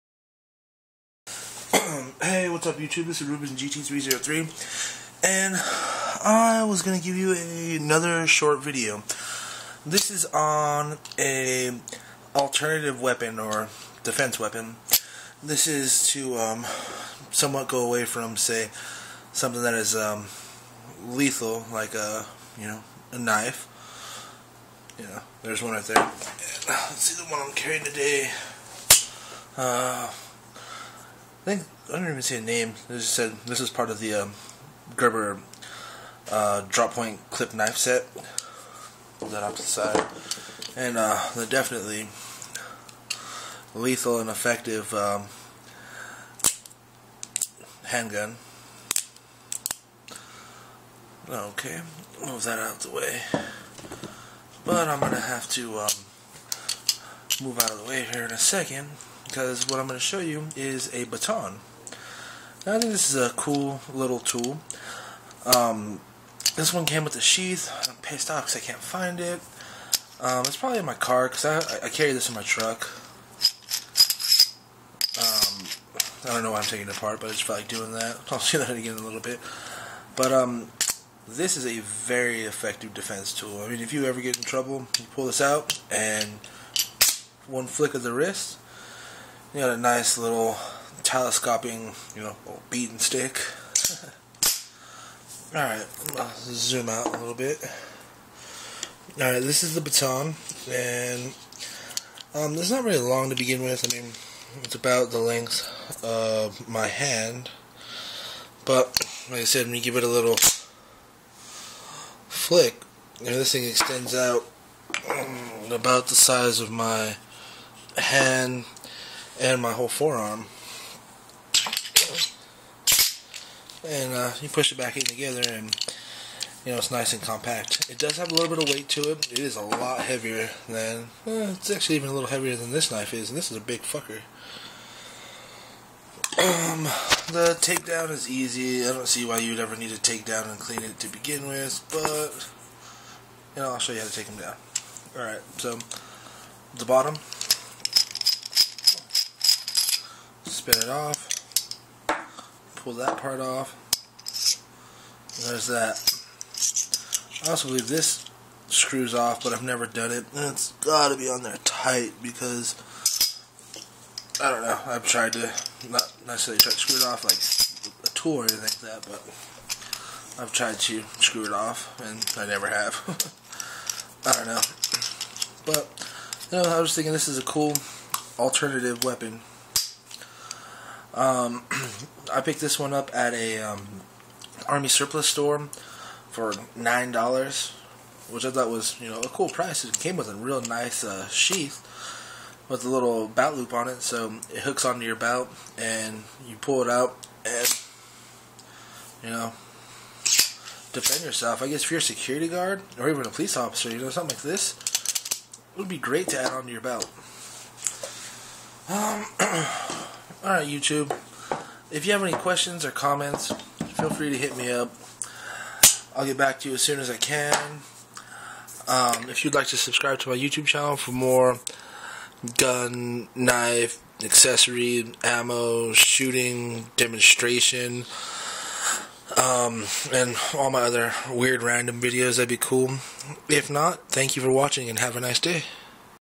Knife And Gun Anternative Weapon
A review of an alternate weapon choice, the collapsable baton